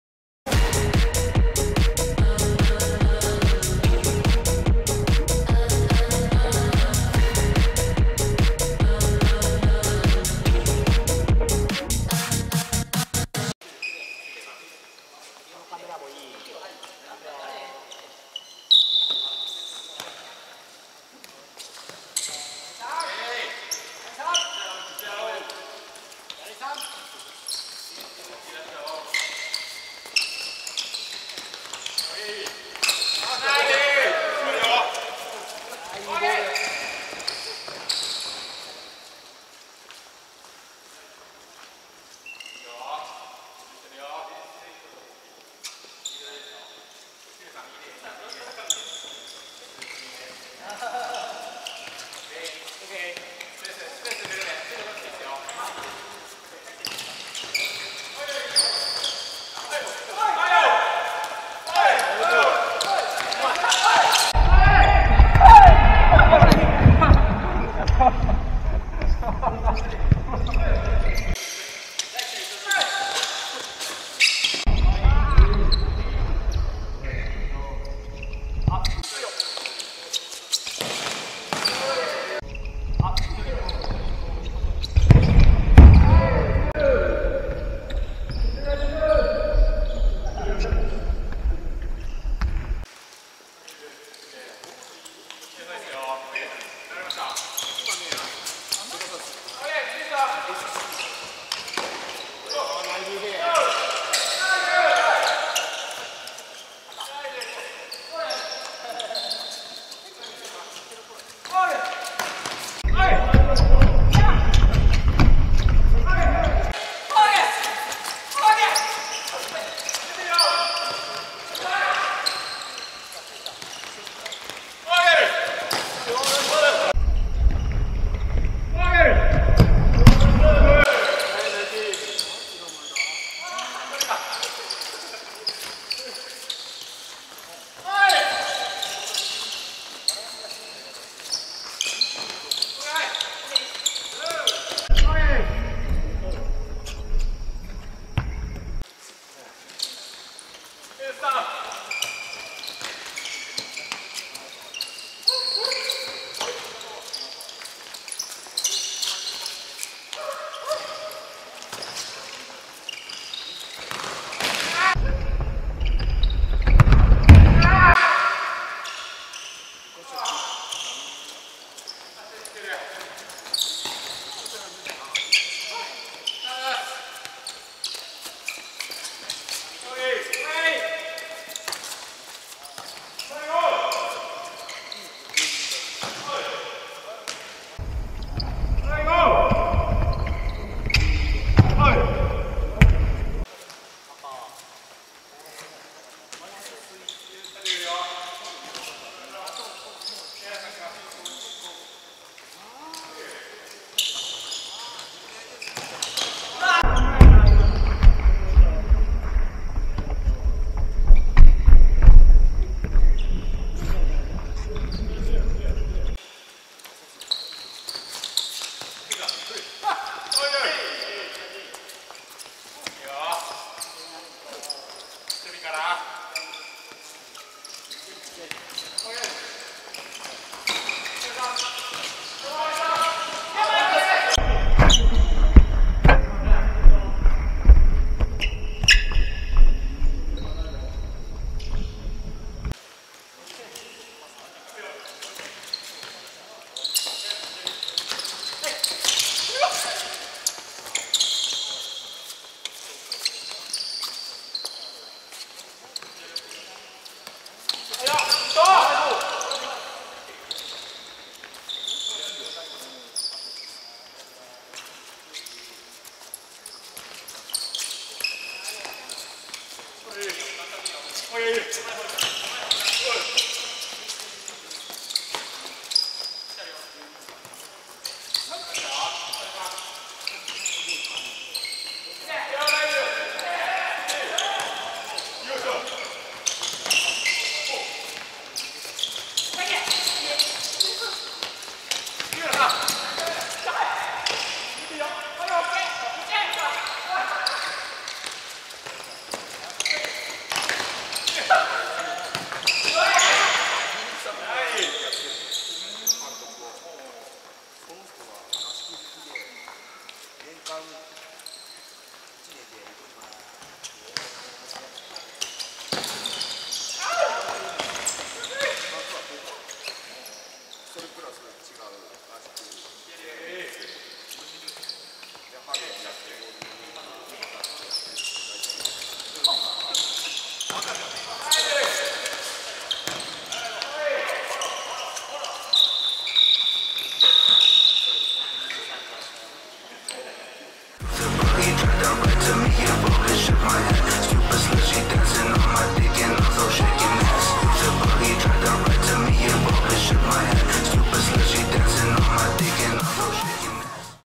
【2023-24】2023.09.10. 5th GAME 『声出して 走る』
９月１０日

３週連続練習の３回目
今日は最初から最後まで
何時もより数倍も早い動きで
全チームが激しく戦って
見所は全試合
見てても、やってても楽しかった

男性12名、女性3名
3チームに分かれて
12ゲームを楽しみました。

興味のある方は是非ご連絡下さい。